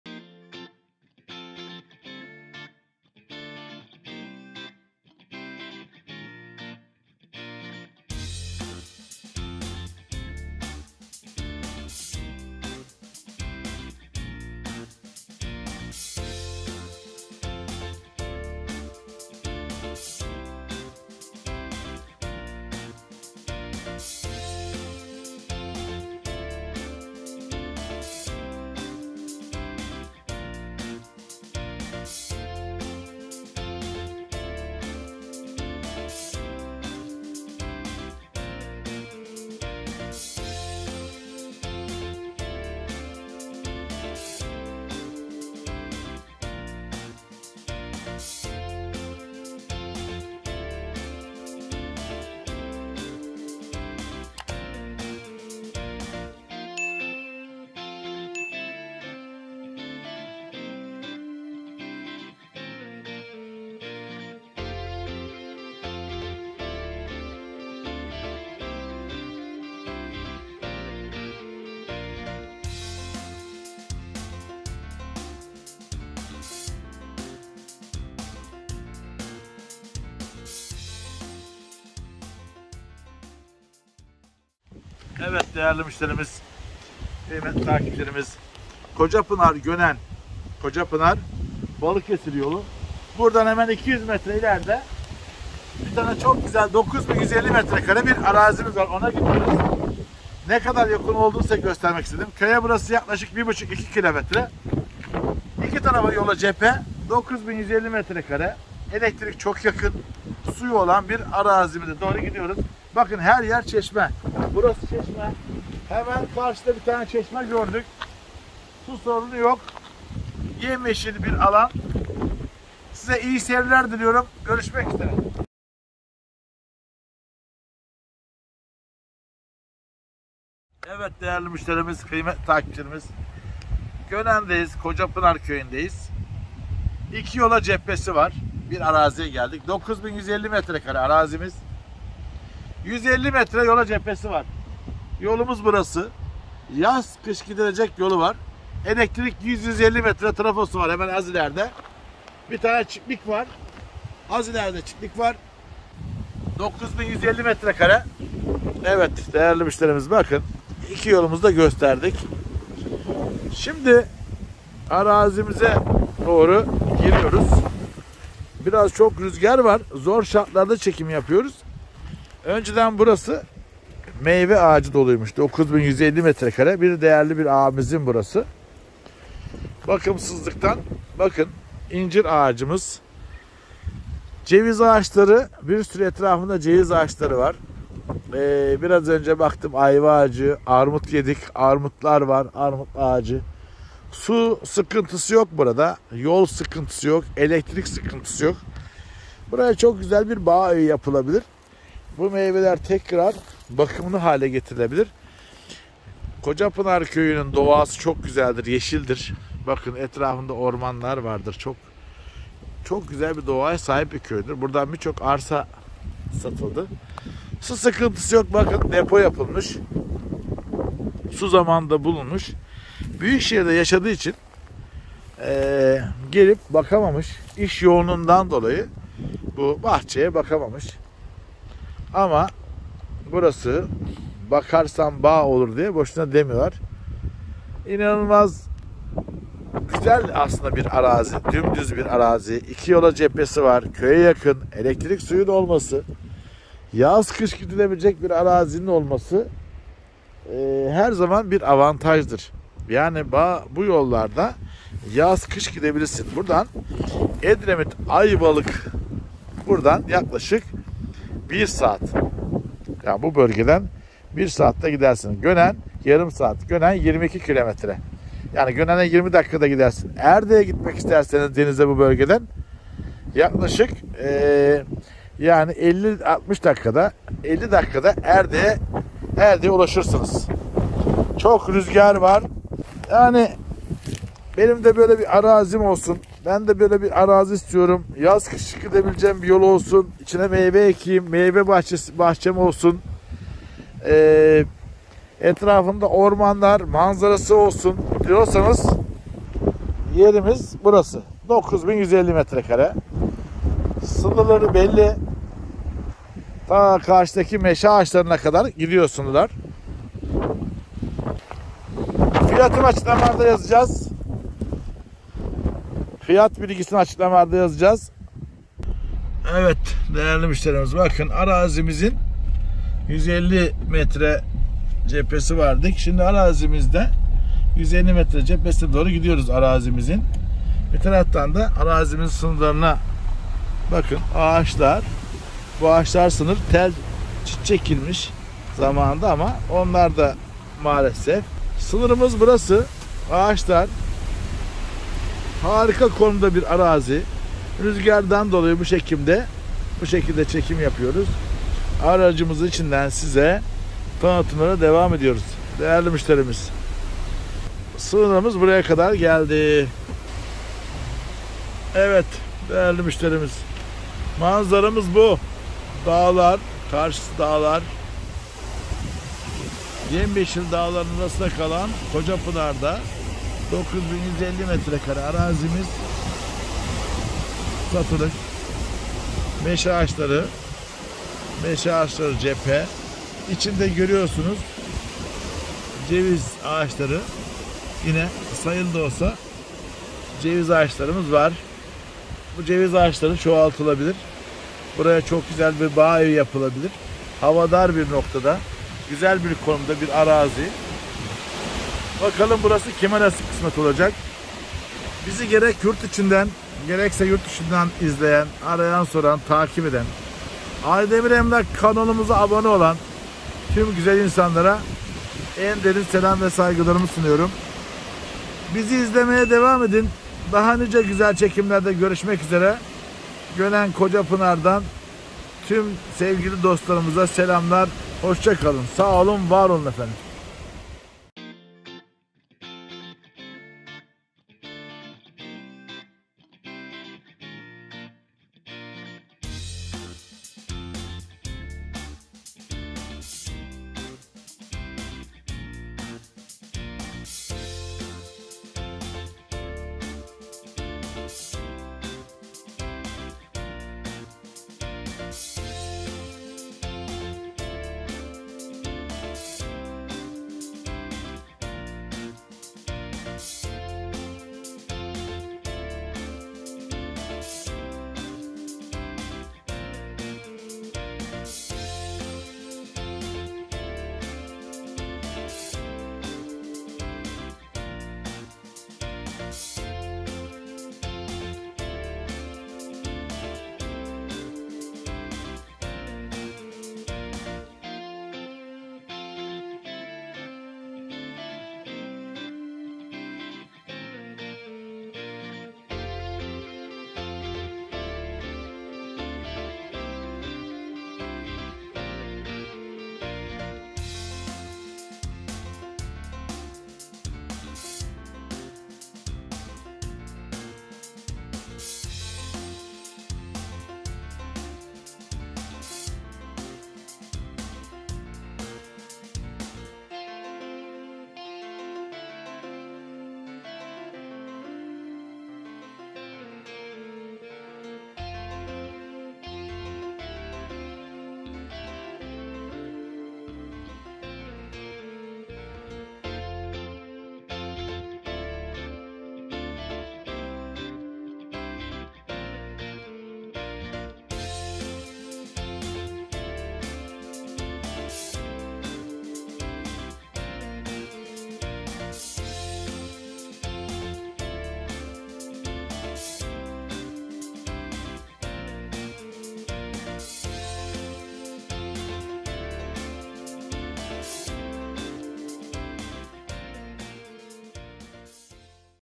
1071- GÖNEN'DE BAĞ EVİNE UYGUN RESMİ YOLA CEPHE 9.150 M2 ARAZİ
FİYAT: 500.000 TL
DETAYLI BİLGİ:
* 9.150 M2
* RESMİ KADASTRO YOLU MEVCUT
* 150 METRE YOLA CEPHESİ MEVCUT
* İKİ YOLA CEPHE
* SU SORUNU YOK
* BAĞ EVİNE UYGUN
* ELEKTRİK TRAFOSU ARAZİYE 100-150 METRE MESAFEDE
* İNCİR, CEVİZ, ARMUT, AYVA VB MEYVE AĞAÇLARI BULUNMAKTADIR
* EDREMİT VE AYVALIK 1 SAAT MESAFEDE
* KÖYE 3 KM, GÖNEN 20 DK

AHMET AYDEMİR: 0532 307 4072
BERKAY AYDEMİR: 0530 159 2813

OFİS ADRES: Malkoç Mah. Kızılay Cad. No:4/C   GÖNEN/BALIKESİR